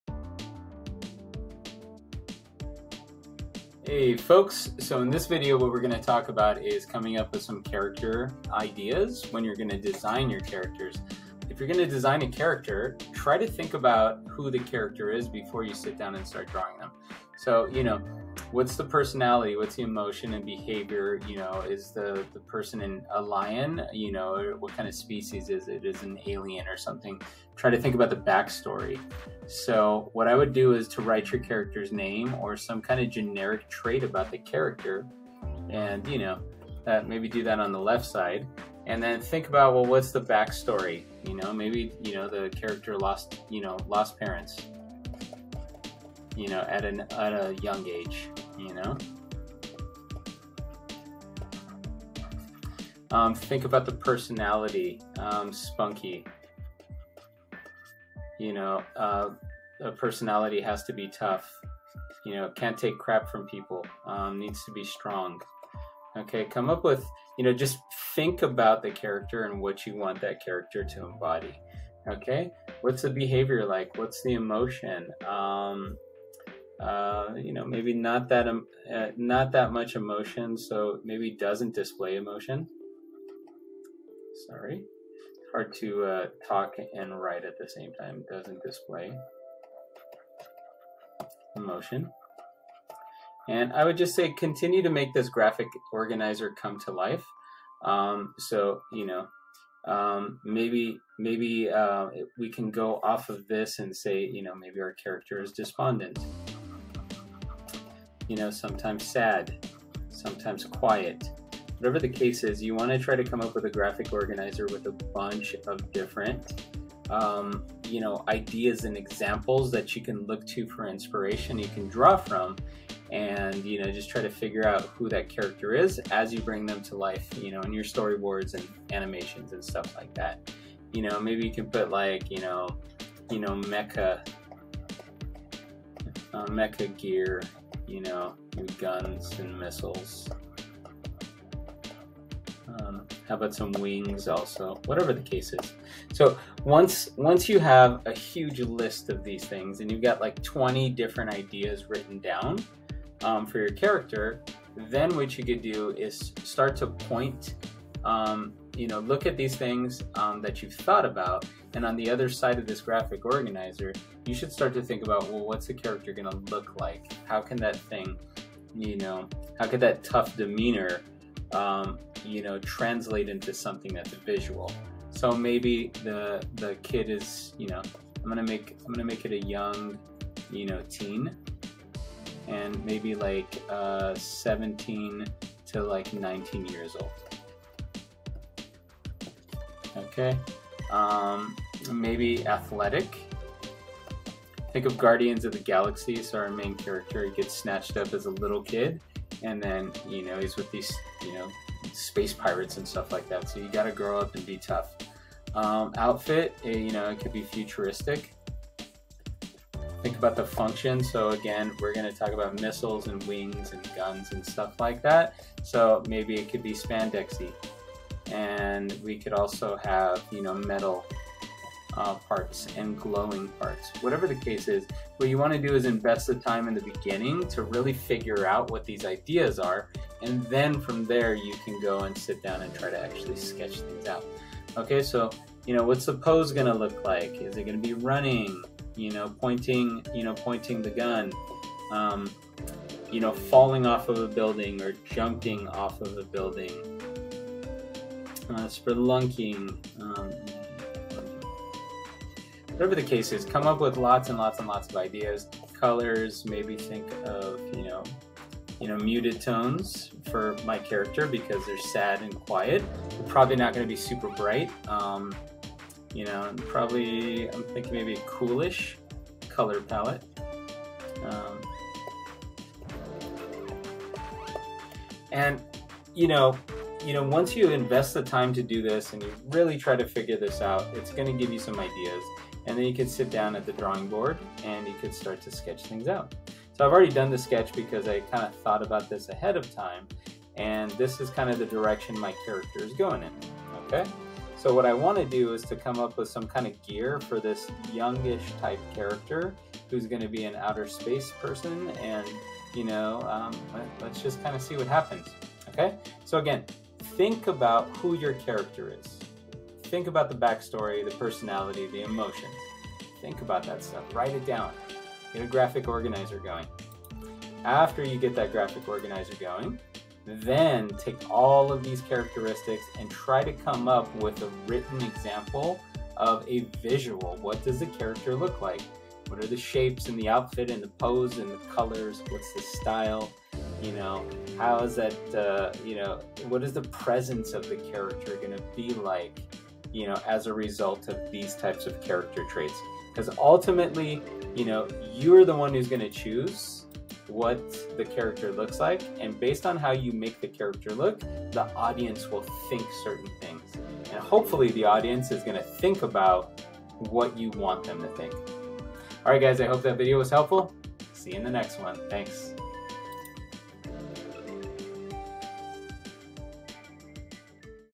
Character Design Ideas
This video covers the thinking and planning that happens behind the scenes when making character designs.

As you create characters, it's a really good idea to think about the character's personality, back story, temperament, job so you can develop the matching aspects of what they look like. Check out Wouter Tulp's class on Schoolism if you'd like to complete an in depth course on character design. In the meantime, give these ideas a shot. I have a feeling your characters will look, and act, in a much more interesting fashion.